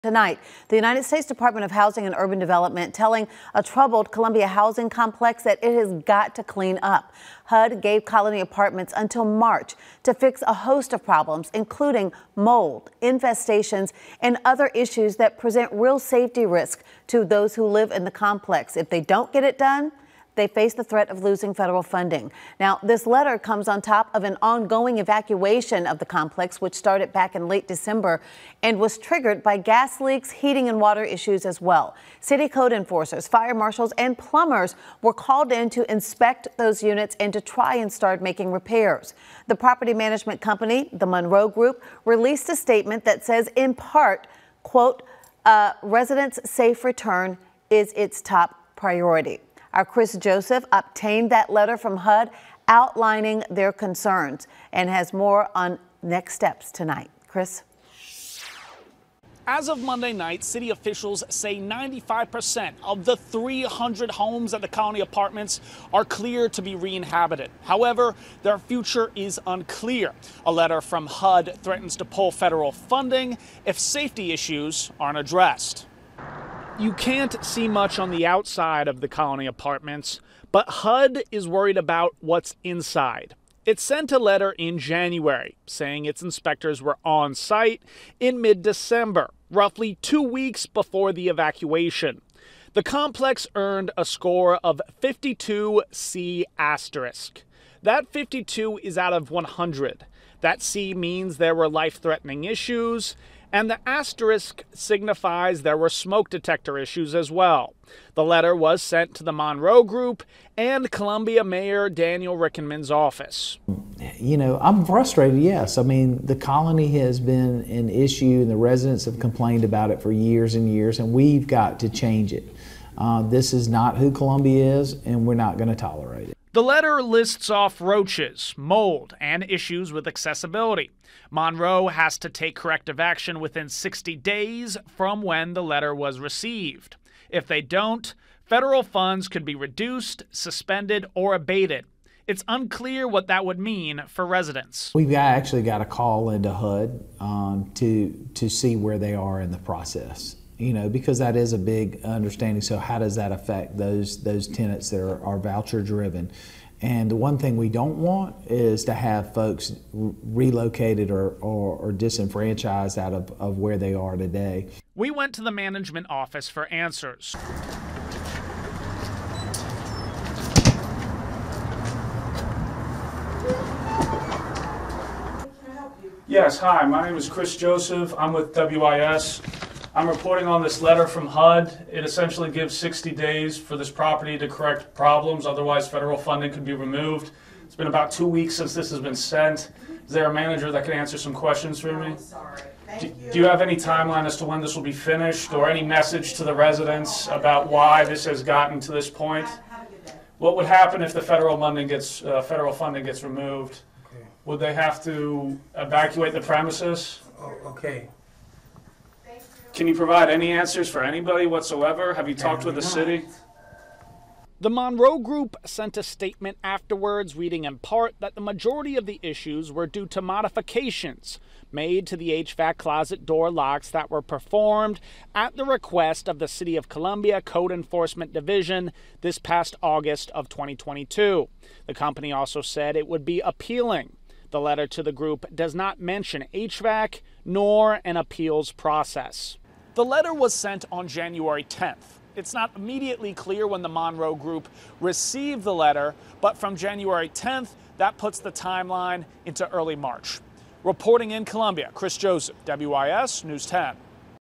Colony Apartments has until March to fix safety issues
The United States Department of Housing and Urban Development (HUD) sent a letter to the ownership of Colony Apartments threatening to pull federal funding if life-threatening health concerns are not addressed.

The letter is dated Jan. 10 and gave the ownership group 60 days after receiving the letter to take corrective action.